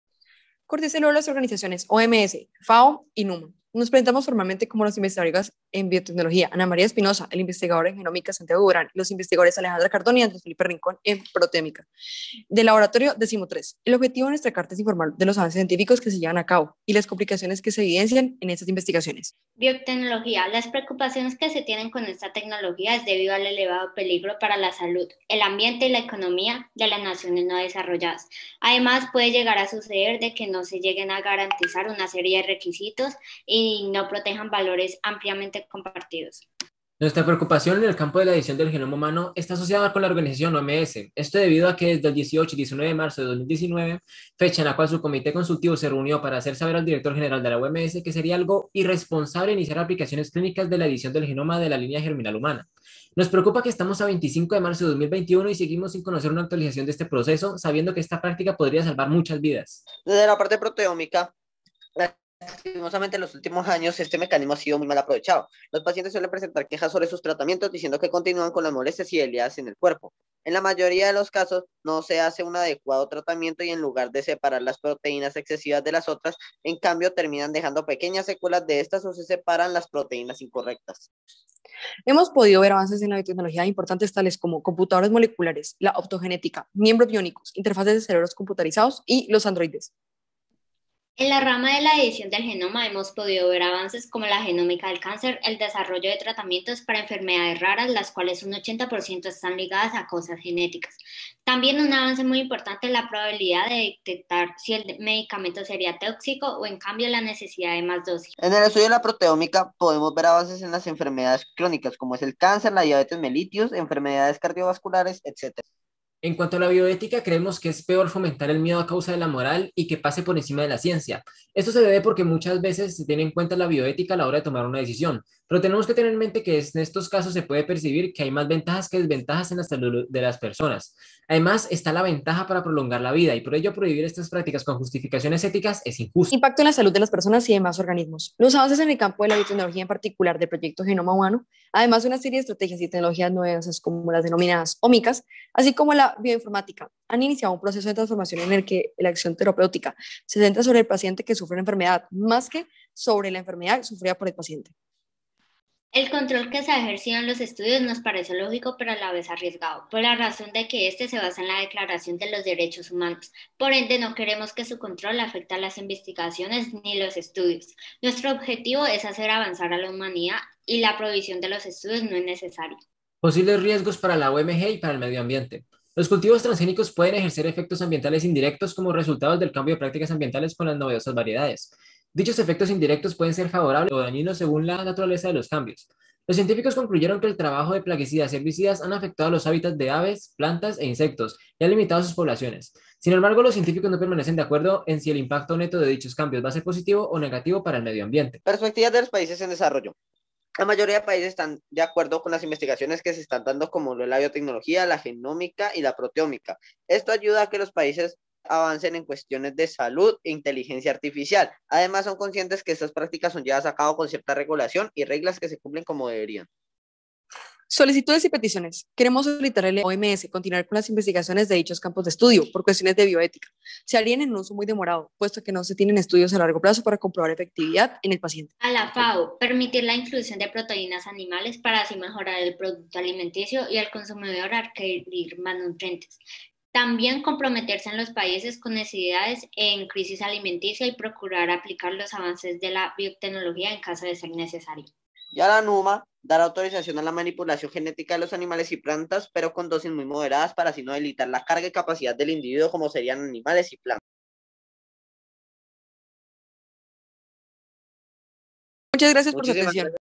Carta dirigidas a las organizaciones OMS, FAO y PNUMA
Este video explica nuestra carta dirigida a todas estas organizaciones